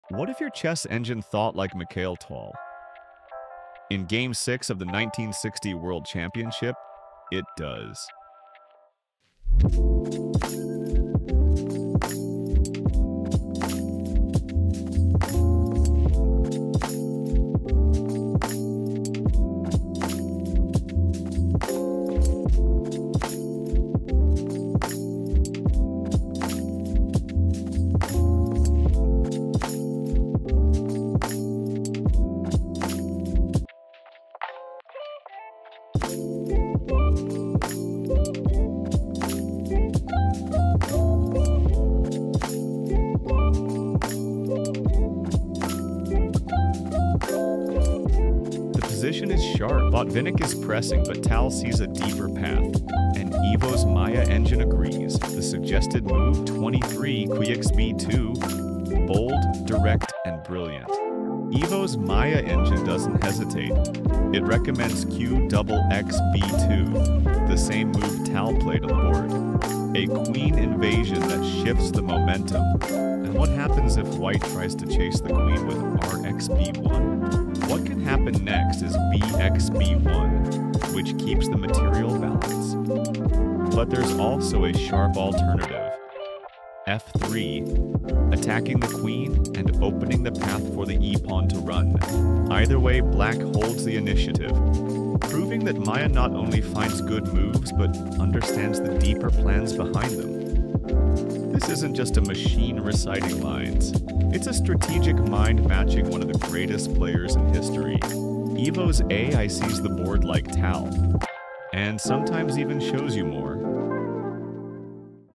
Maia Matches Tal’s Tactical Eye#chess
Imagine if your chessboard assistant could think like Mikhail Tal? ♟️

In Game 6 of the 1960 World Championship, Tal boldly played 23...Qxb2—and Evo’s Maia engine on Level 9 recommended the exact same move! 🔥

In this video, you’ll discover:
👀 How Maia predicts Tal’s tactical vision
⚡ What sharp variations arise if White chases the queen
♟️💡 Why Maia doesn’t just find “good moves” but understands deep strategic ideas

This is more than chess—it’s a collision of AI and a historical master’s mind!